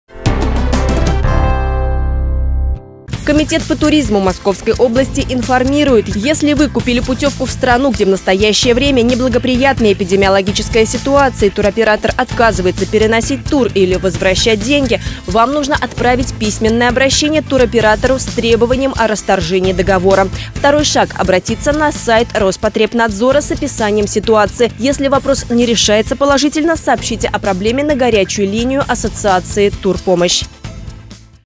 Вниманию туристов
Комитет по туризму Московской области информирует. Если вы купили путёвку в страну, где в настоящее время неблагоприятная эпидемиологическая ситуация и туроператор отказывается переносить тур или возвращать деньги, вам нужно оправить письменное обращение туроператору с требованием о расторжении договора.